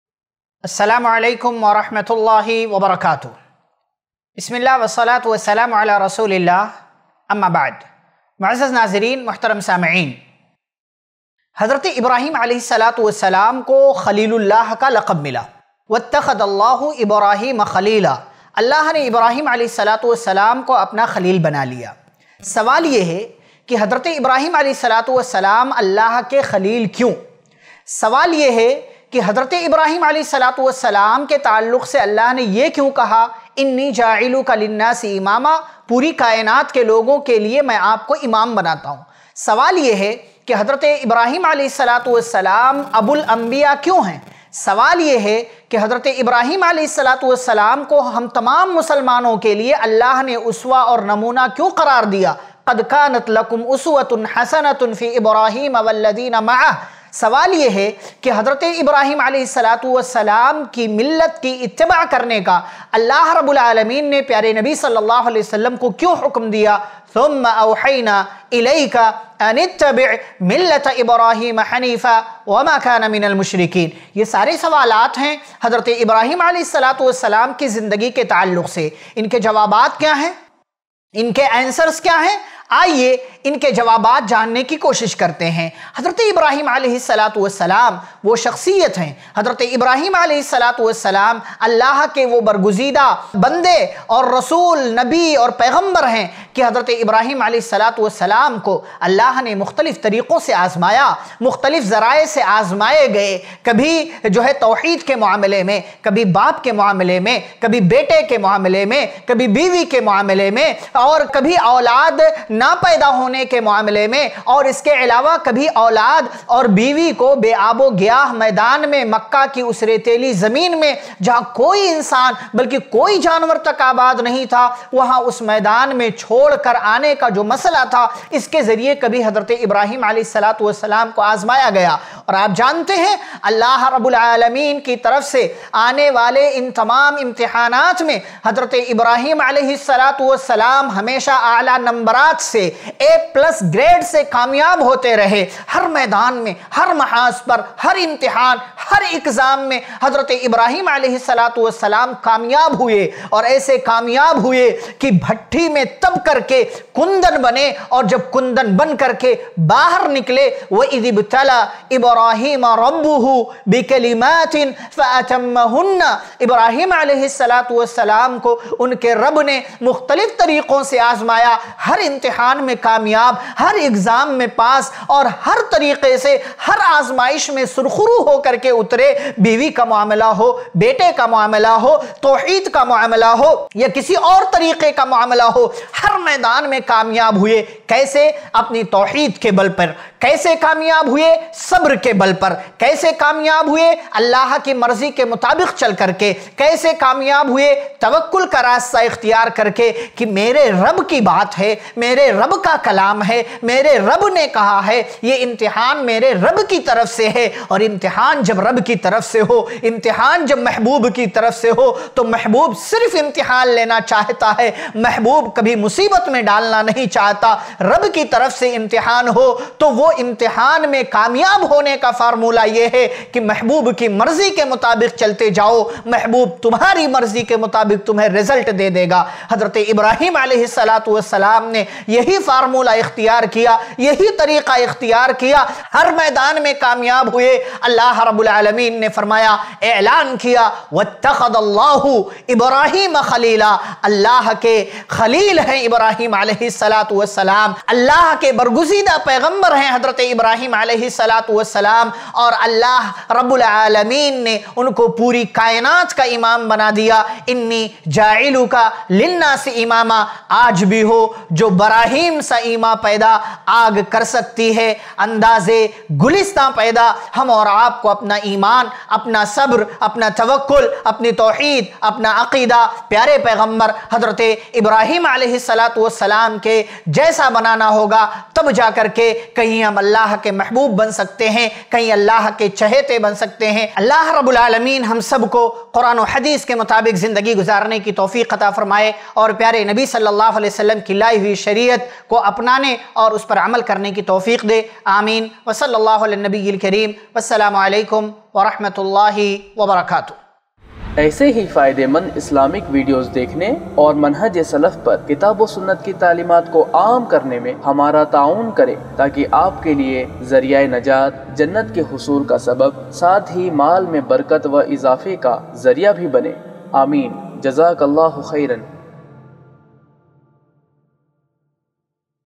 Hazrat Ibrahim A.S. Allah Ke Khalil Kyun ? | Quran Ki Baatein Ep. 14 | Khalilur Rahman Sanabili
Bank Account Details:
ILM FOUNDATION
ICICI Bank
Savings account
IFSC CODE-ICIC0001028

Hazrat Ibrahim Alaihissalam Allah Ke Khalil Kyun? | Quran Ki Baatein Ep. 14 | Hafiz Khalilur Rahman Sanabili | IIC Mumbai